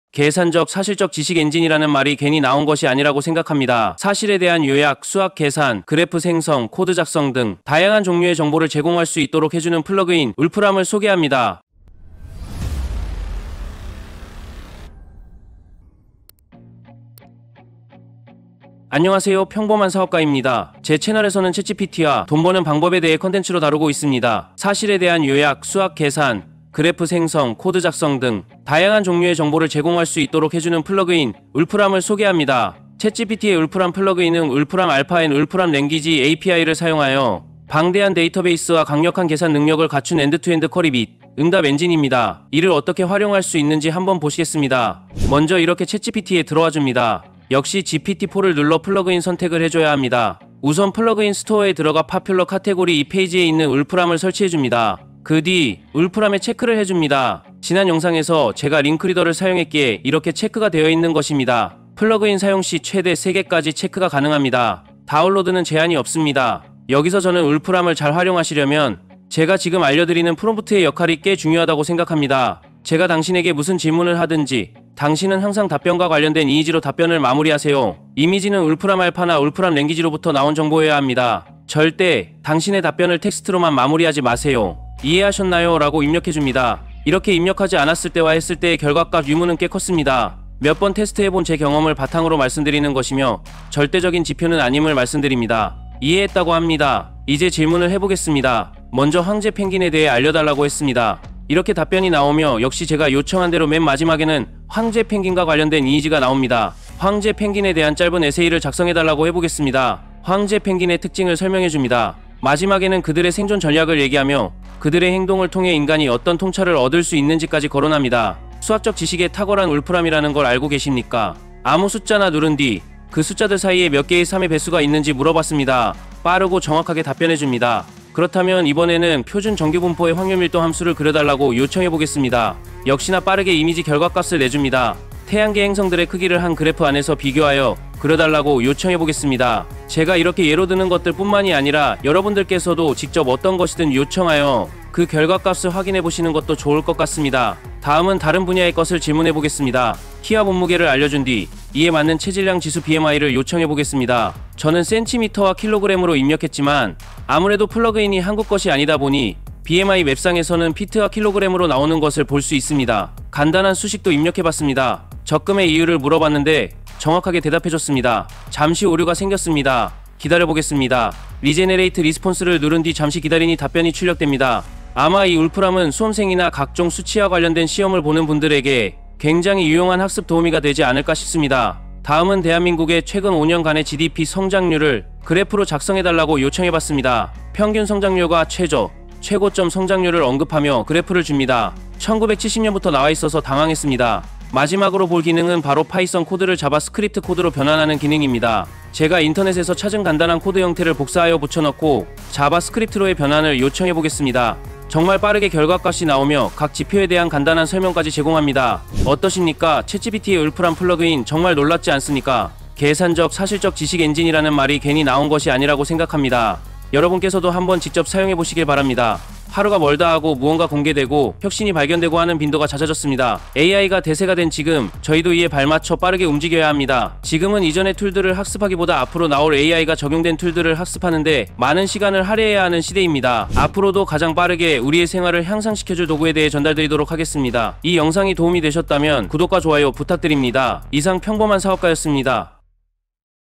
Wolfram alpha 수학이 가능하여 그래프까지 그려주는 엄청난 ChatGPT 플러그인.
📝Summary
울프람알파 Wolfram alpha 플러그인은 챗GPT와 함께 사용되며, Wolfram Alpha & Wolfram Language API를 사용하여 다양한 정보와 강력한 계산 능력을 제공합니다.
🔍 울프람알파 플러그인은 챗GPT와 함께 사용되며, Wolfram Alpha & Wolfram Language API를 사용하여 다양한 정보와 강력한 계산 능력을 제공합니다.
💡 Wolfram은 수학적인 계산 뿐만 아니라 다양한 분야의 정보를 제공하며, 수험생이나 수치와 관련된 시험을 준비하는 사람들에게 유용한 도구로 활용될 수 있습니다.
🌐 Wolfram 플러그인은 파이썬 코드를 자바 스크립트 코드로 변환하는 기능과 최근 5년간의 GDP 성장률을 그래프로 작성하는 기능을 제공합니다.

⏰Time stamp
00:00  하이라이트
00:31  울프람 알파 Wolfram alpha 플러그인 소개
01:26  프롬프트 설명
01:38  질문 예시 시작
01:50  황제 펭귄 에세이 요청
02:06  3의 배수 개수 질문
02:16  태양계 행성 크기 비교
02:31  체질량 지수와 수식
02:55  Wolfram의 학습 도움
03:05  GDP 성장률 그래프
03:15  파이썬 코드 자바스크립트로 변환